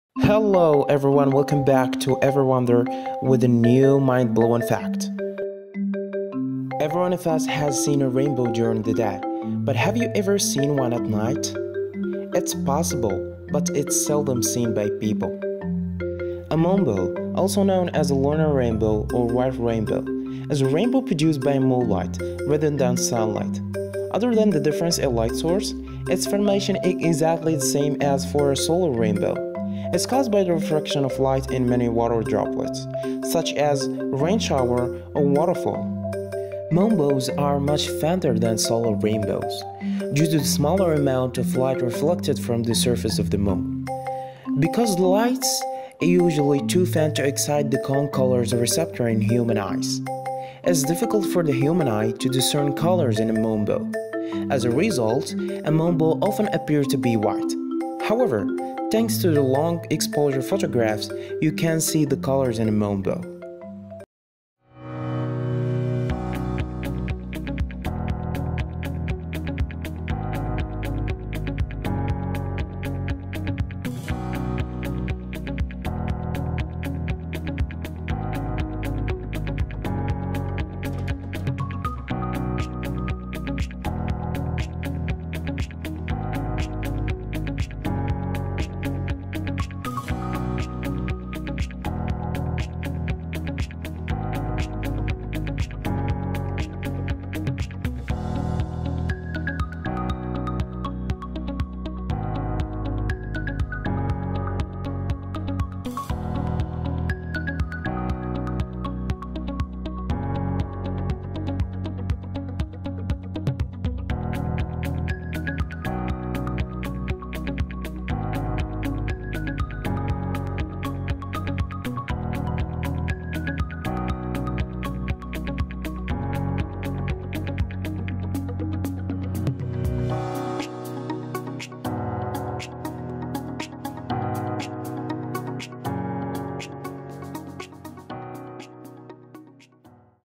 Moonbow- Light from the moon can create rainbows
Everyone loves a good rainbow, but did you know that Light from the moon can create rainbows, called moonbows we can see them at night, too? What are moonbows and how do they work?